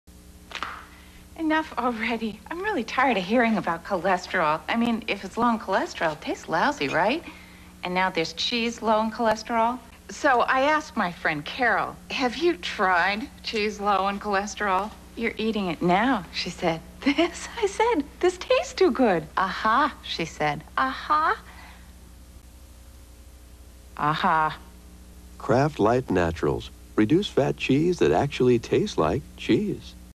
Kraft - Light Naturals - Reduced Fat Cheese - Low Cholesterol Commercial (1990)
Transferred from a VHS recording of the ABC Monday Night Movie  Broadcast of "Jewel of the Nile" on Monday February 12, 1990